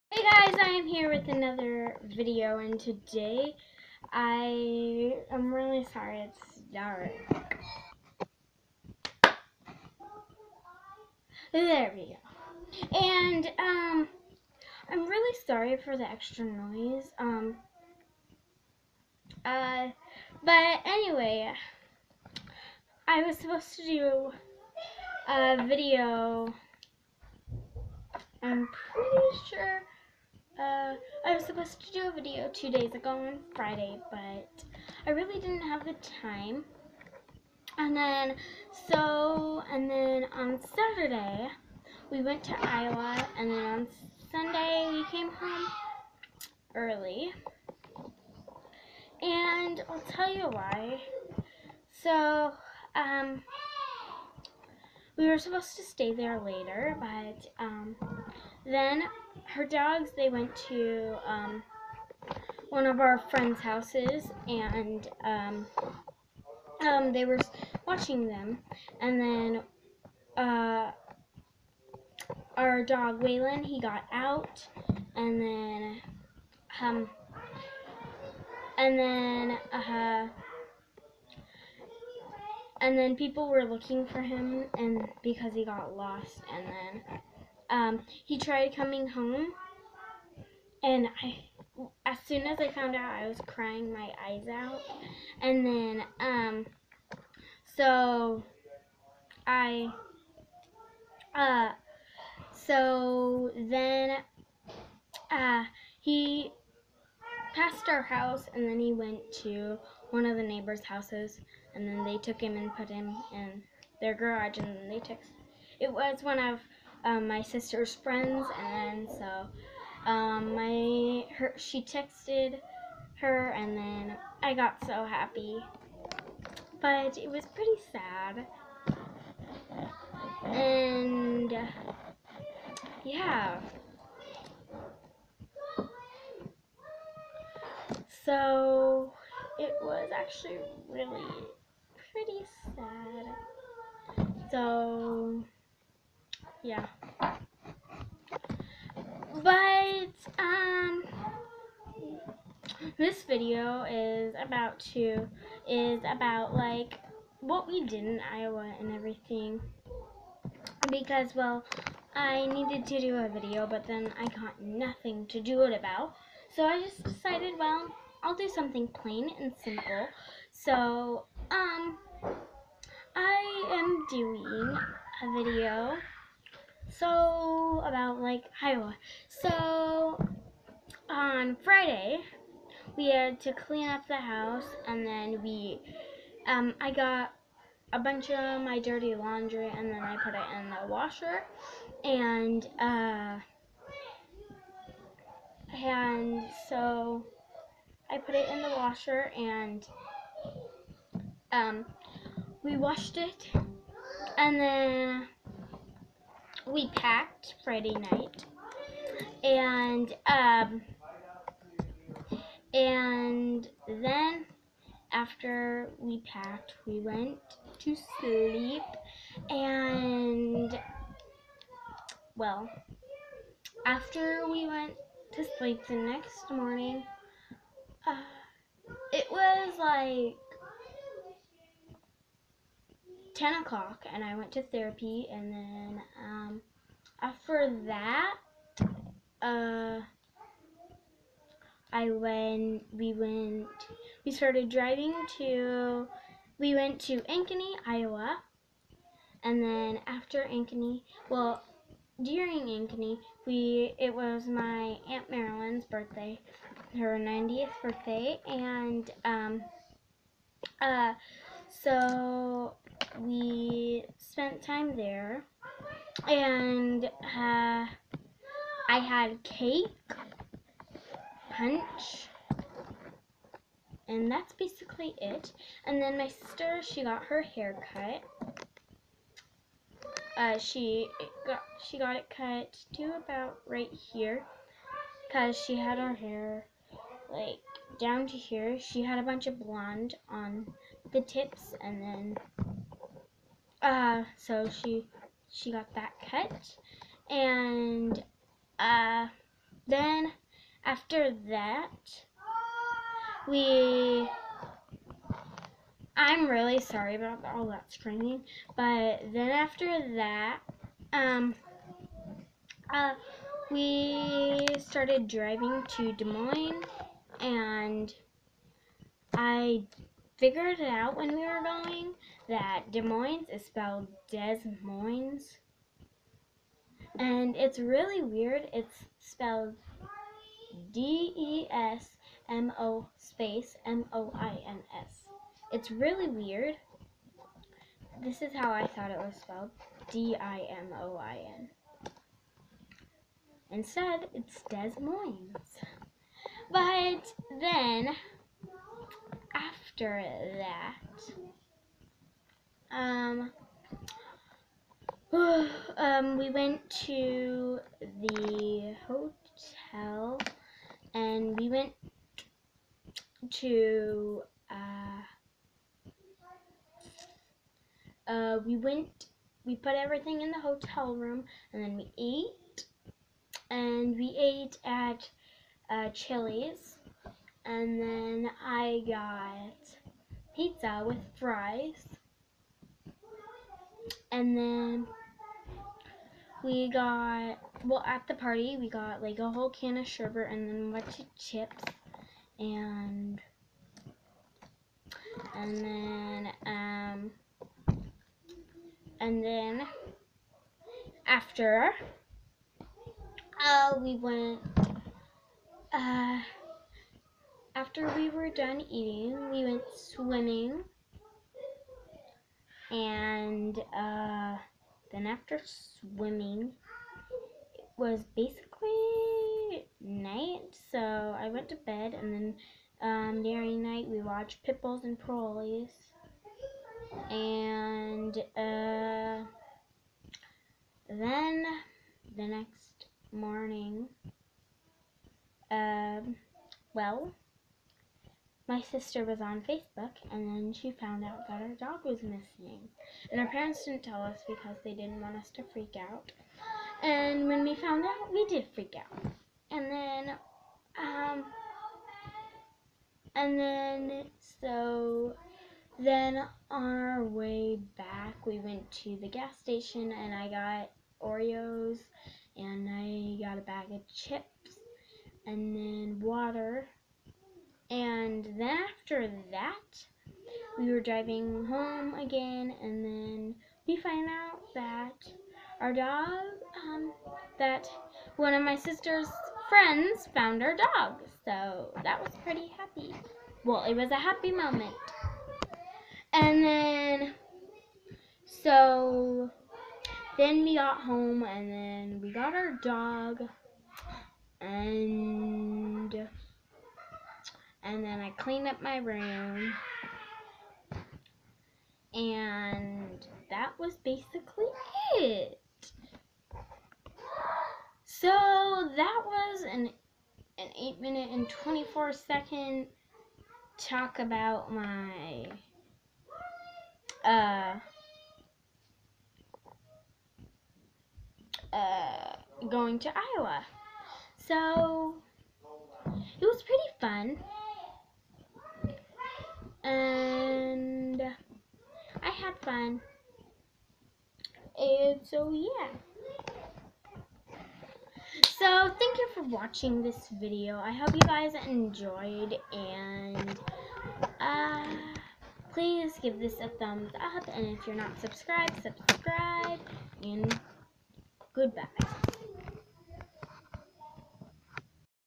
LOST DOG! O know!
cried my eyes out when I found out our dog got lost!